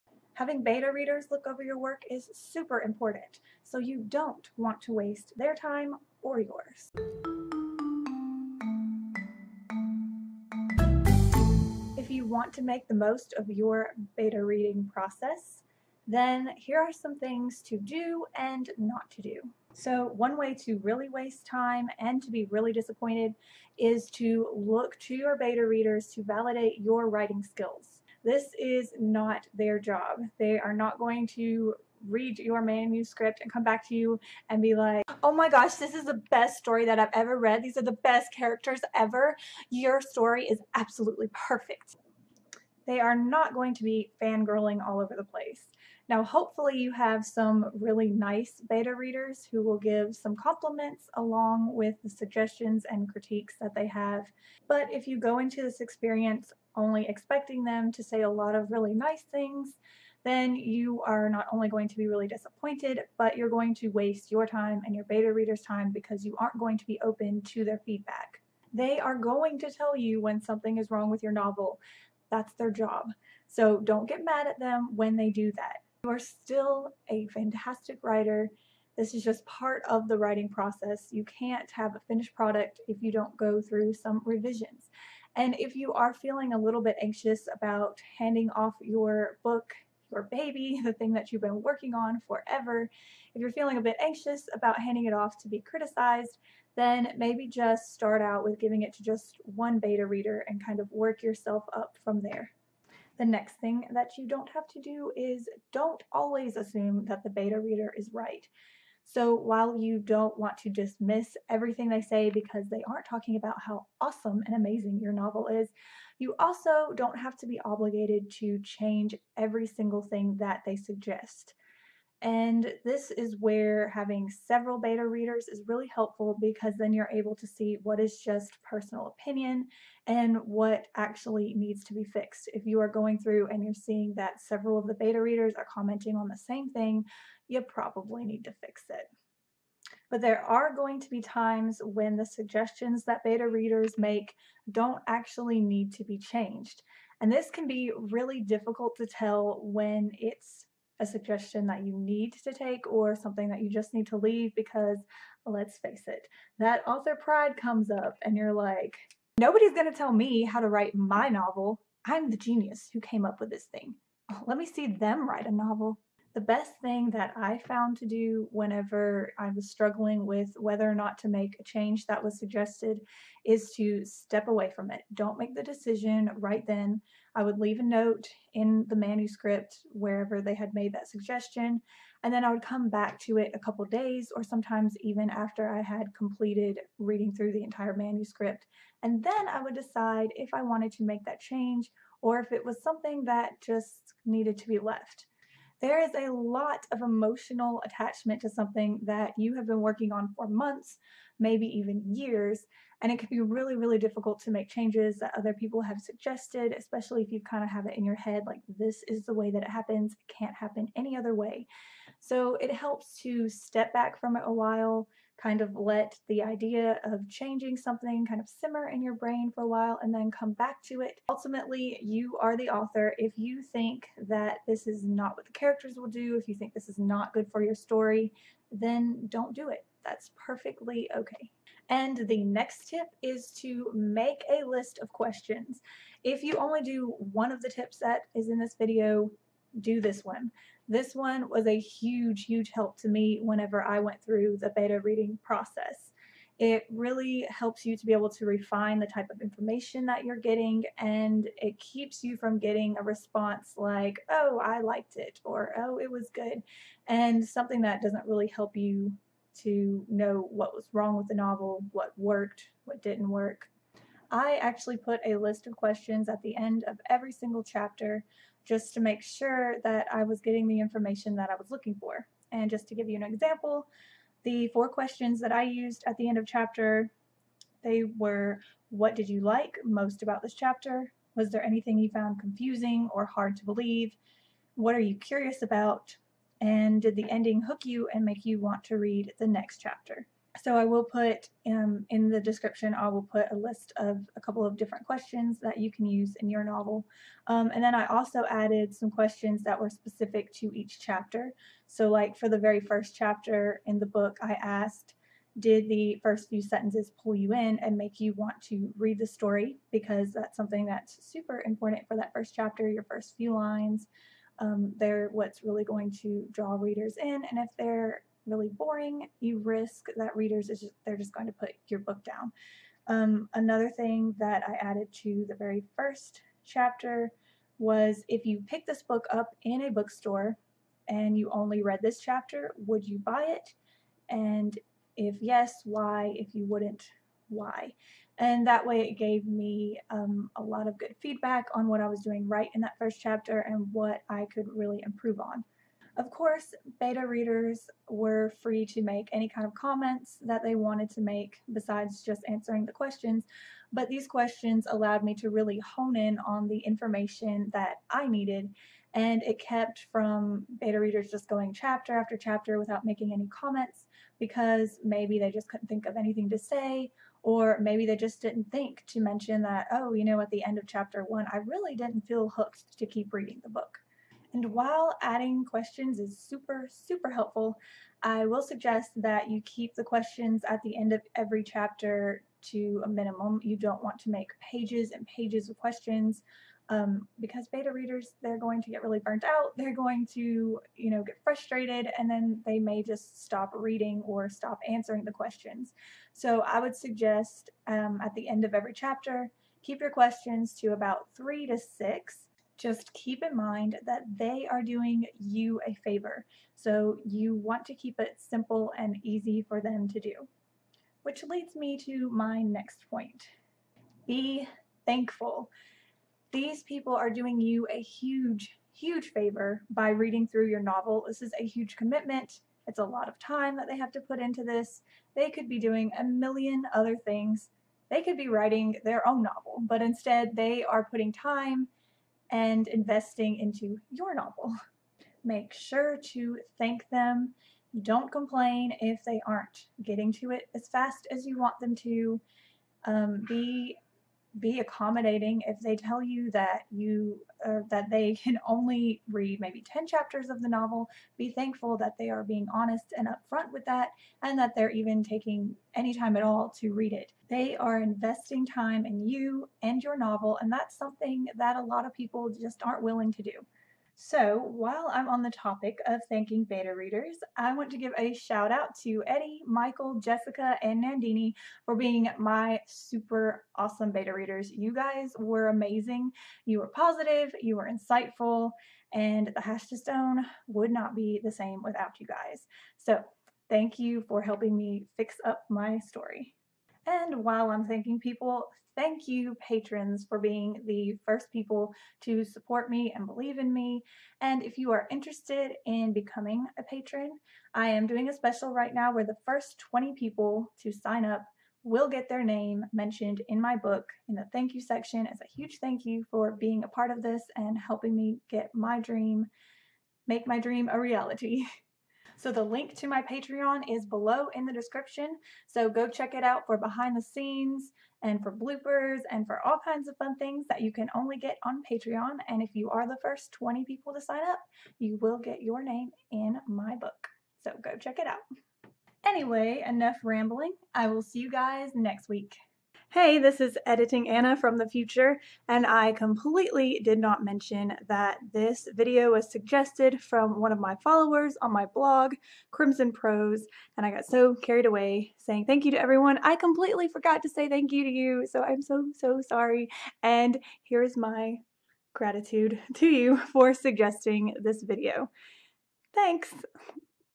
Getting the Most out of Beta Readers (Fox Five Extra)
You've put a lot of effort into finding people to read your book, so don't let that effort be wasted! Here are my tips for getting the most out of beta readers.

My list of questions at the end of each chapter:

• What did you like most about this chapter?
• Was there anything you found confusing or hard to believe?
• What are you curious about?
• Did the ending hook you and make you want to read the next chapter?

In the first chapter, I asked:

• Did the first few sentences pull you in and make you want to read the story?

• If you picked this book up at a bookstore and read only this chapter, would you buy it? If not, what could be changed to make you want to keep reading?

Additional questions:

• Who was your favorite character, and why do you like them?
• Who is your least favorite character, and why don’t you like them?
• What do you wish there was more of in this story?
• Where you bored during any part of this chapter?